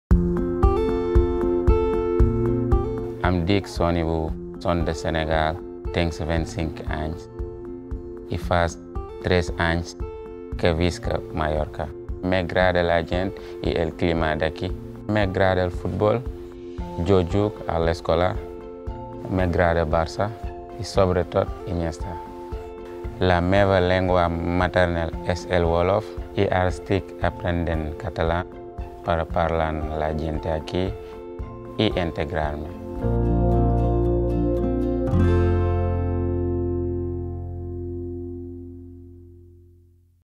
Històries del català a Capdepera: Sonyibou, del Senegal
Em dic Sonyibou, som del Senegal i fa tres anys que visc aquí.
Estic aprenent català per parlar amb la gent d'aquí i integrar-me.